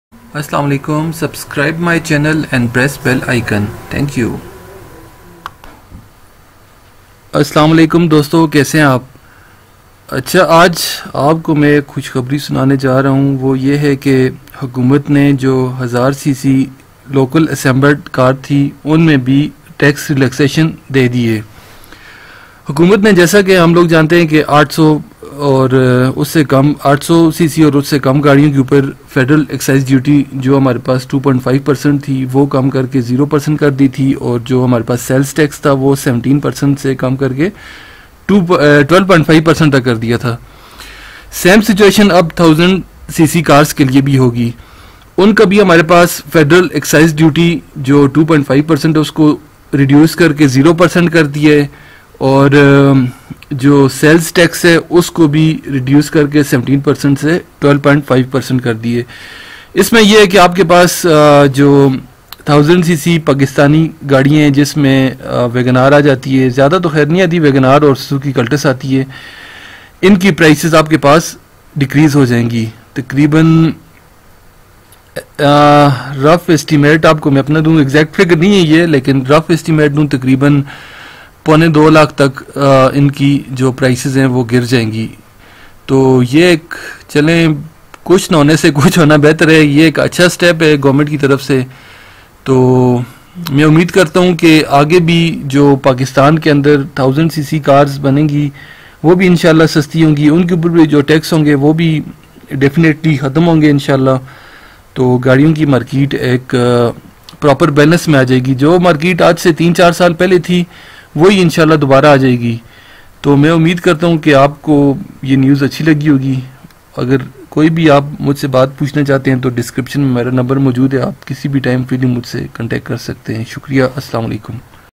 Government reduces 1000cc car prices | New Reduction in car prices | حکومت نےگاڑیاں مزید سستی کر دیں
Finally Government announces to reduce 1000 cc car prices